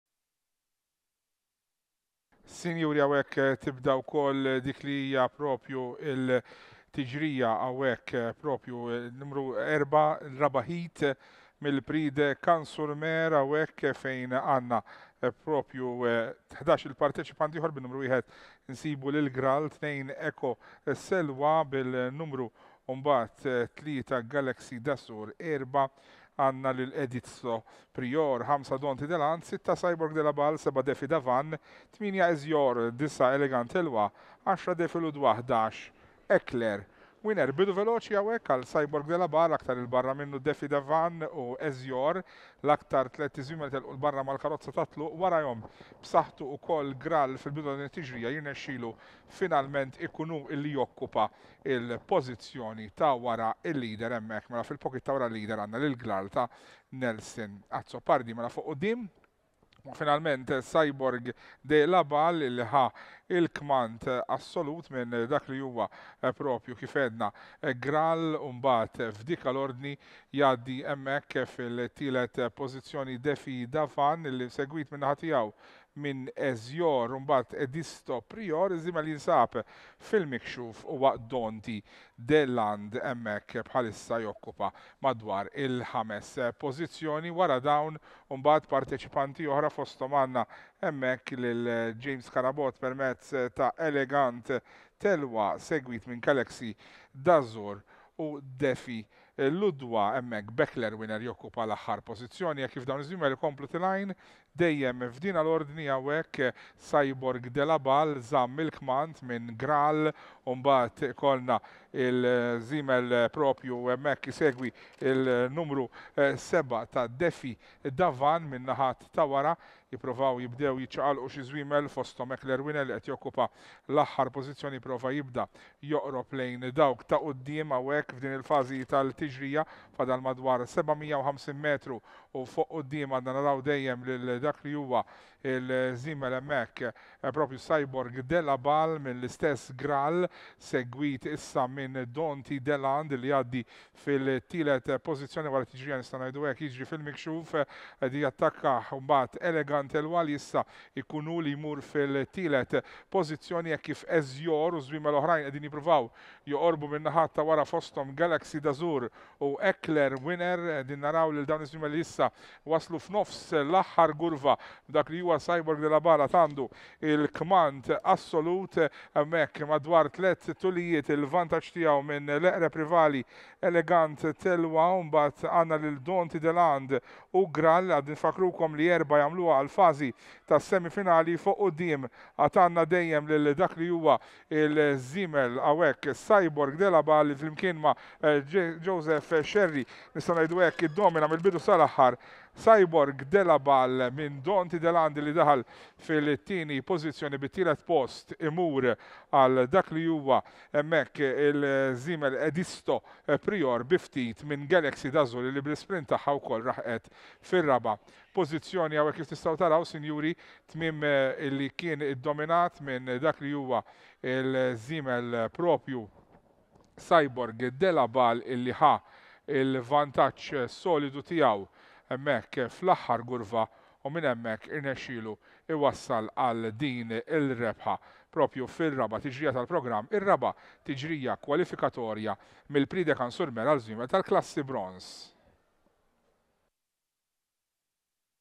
Meeting 24 - 12th May 2023 - Race 4
DUNTY DES LANDES [Silvio Schembri] - Prix de Cagnes Sur Mer Heat 4 - Class Bronze